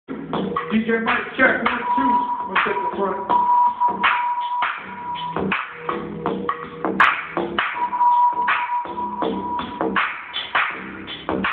Kool Keith Tour Vlog 6
Kutmasta Kurt checks the mics...but where is Keith?